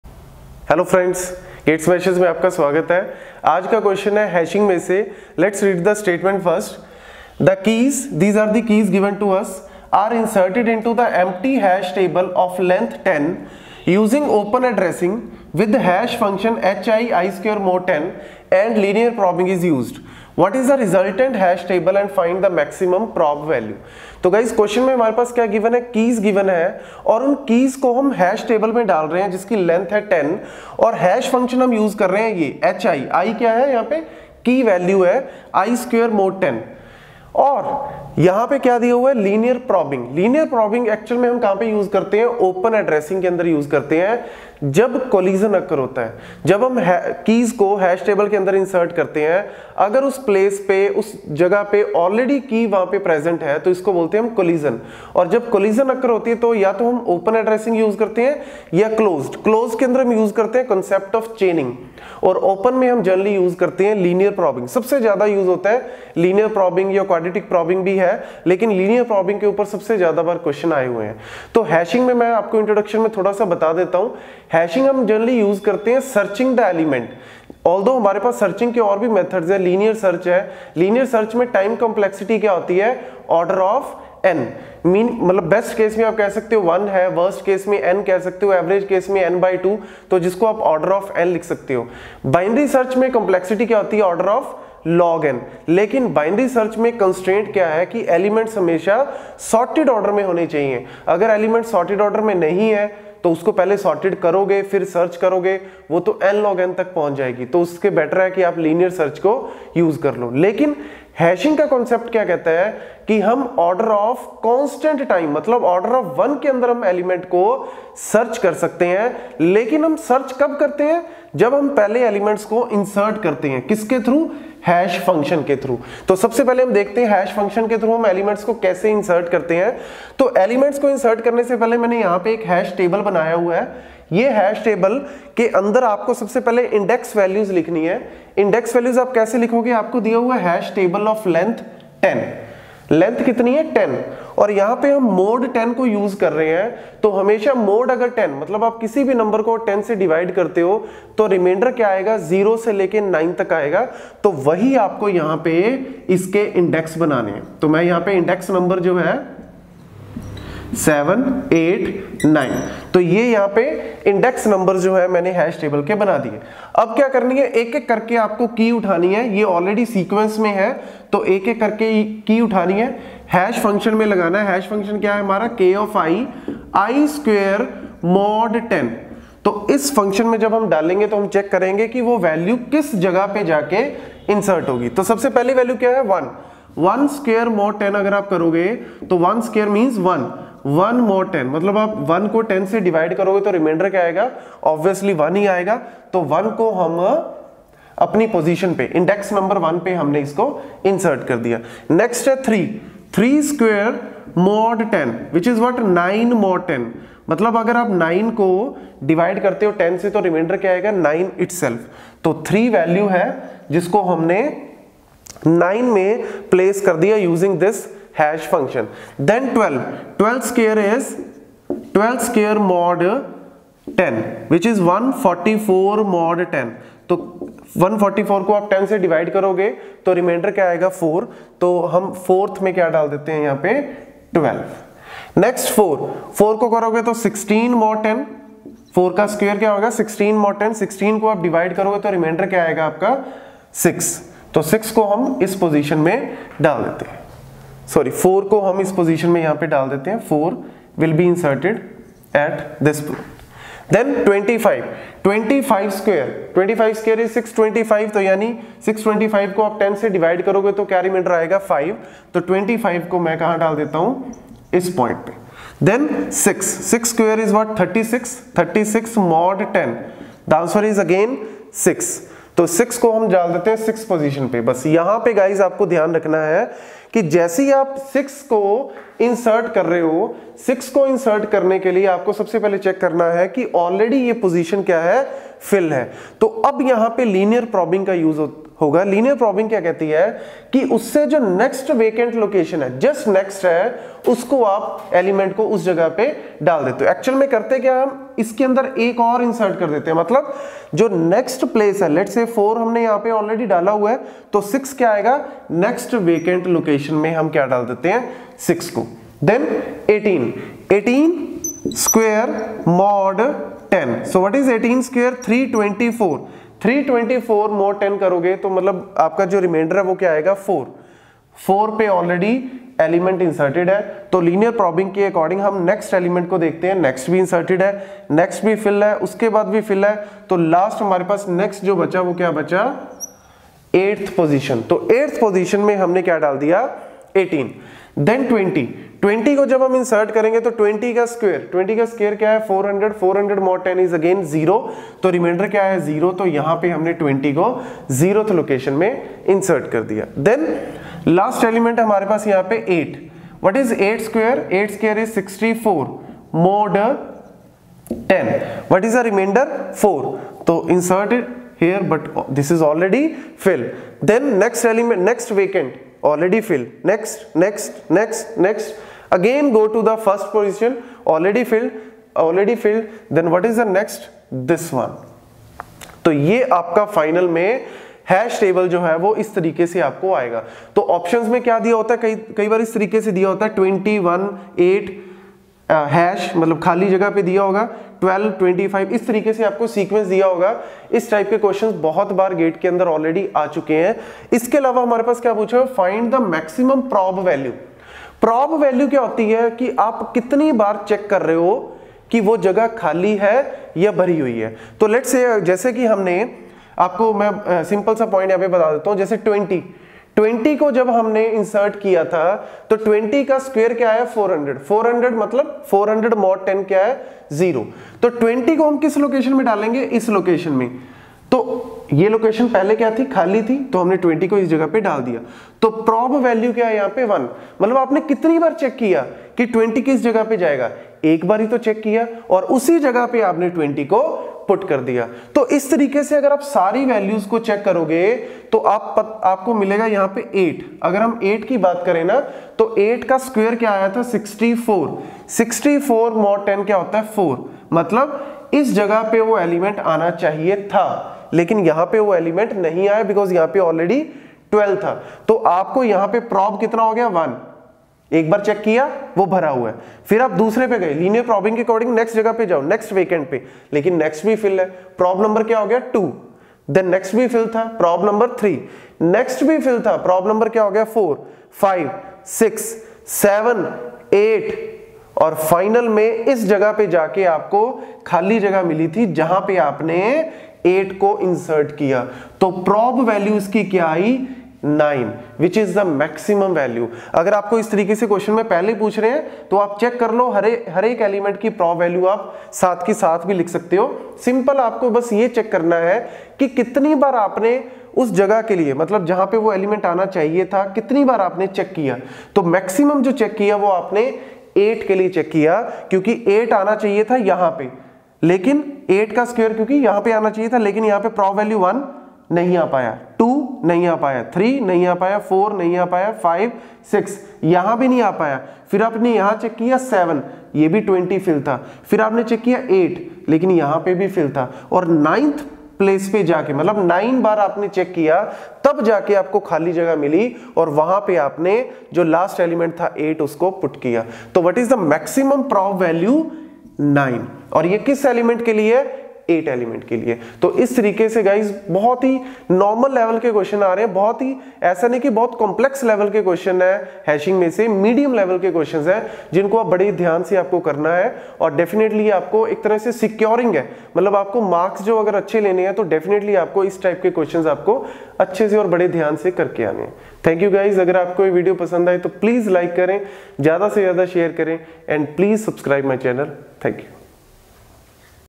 L-6.5: Imp Question on Hashing | Linear Probing for Collision in Hash Table | GATE Questions
In this video, Varun sir will be discussing the important questions on Hashing. It will be helpful for you in the competitive examinations and will clear your concepts in a simplest way possible